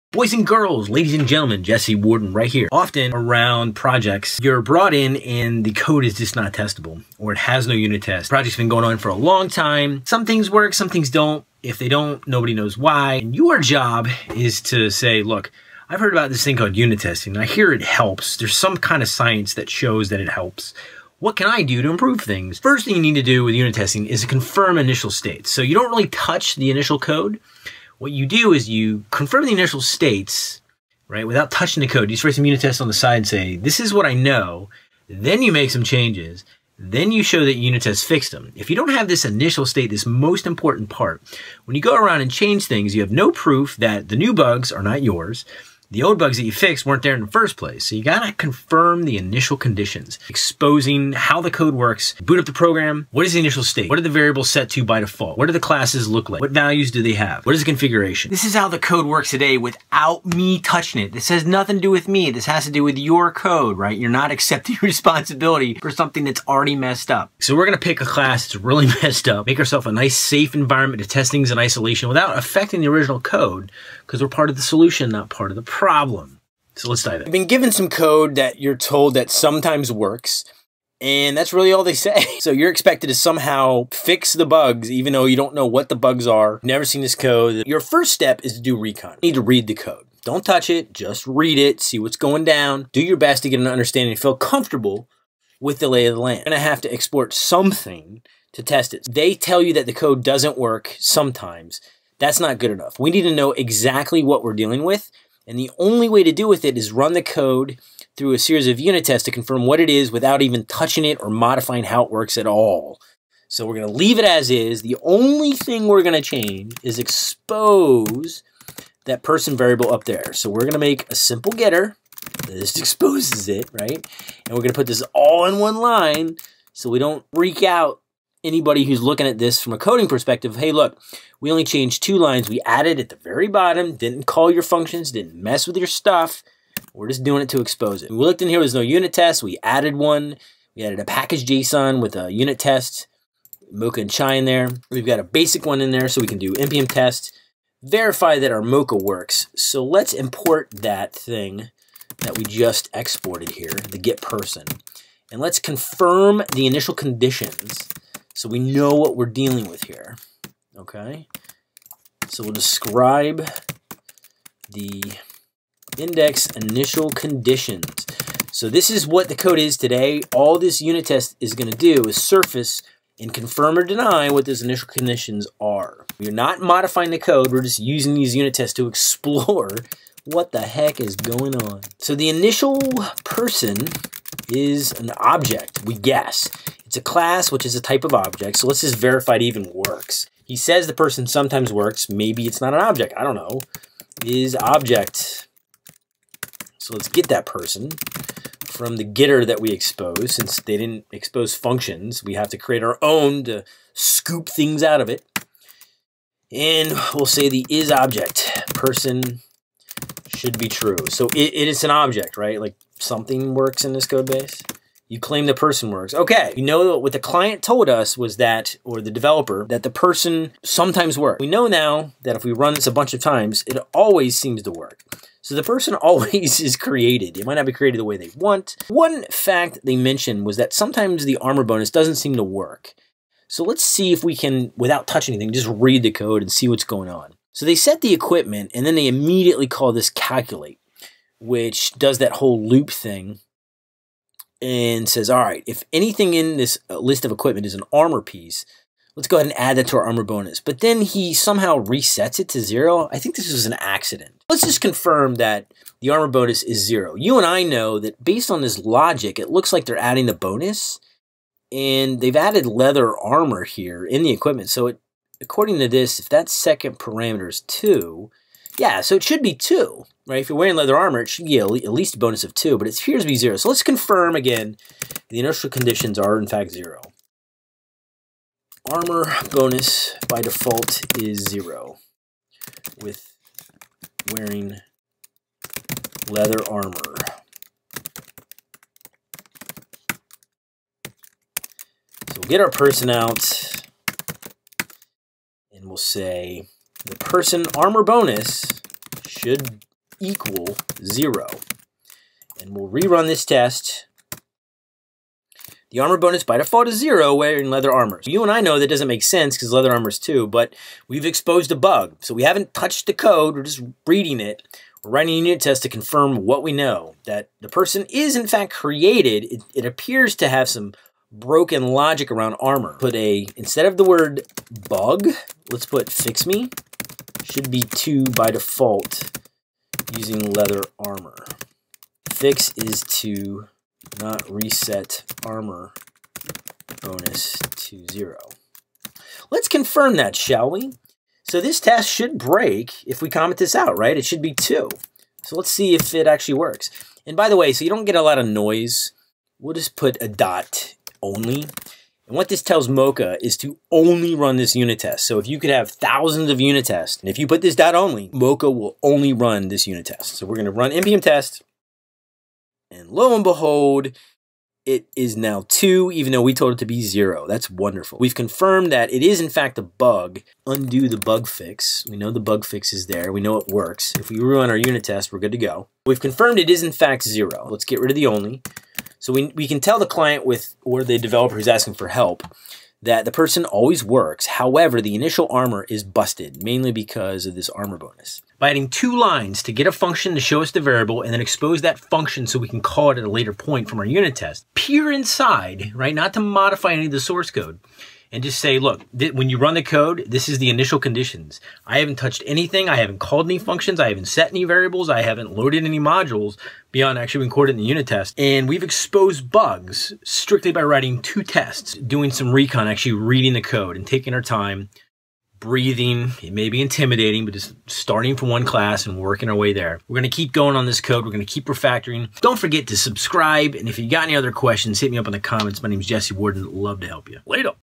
Unit Testing Initial Conditions
I cover unit testing untestable code, and first ensuring you know for a fact what the initial conditions are. You create a safe environment to confirm what things ACTUALLY ARE with factual tests. Then you can change things with confidence in knowing you’re part of the solution and not part of the problem.